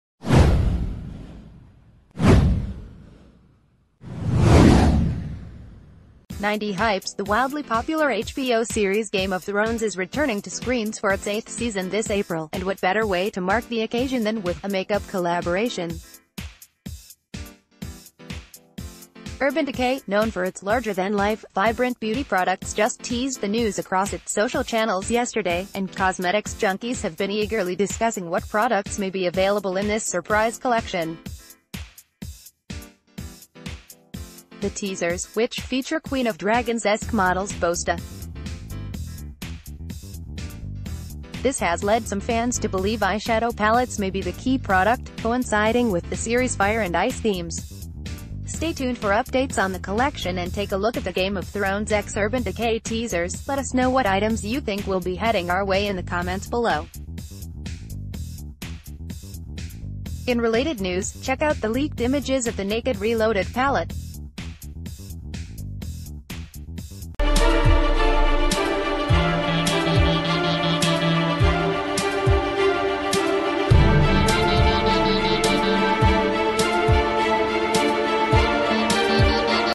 'Game of Thrones' Fans Will Love This New Urban Decay Collab
90 Hypes The wildly popular HBO series Game of Thrones is returning to screens for its eighth season this April, and what better way to mark the occasion than with… a makeup collaboration. Urban Decay, known for its larger than life, vibrant beauty products just teased the news across its social channels yesterday, and cosmetics junkies have been eagerly discussing what products may be available in this surprise collection. The teasers, which feature Queen of Dragons-esque models boast a ForThe...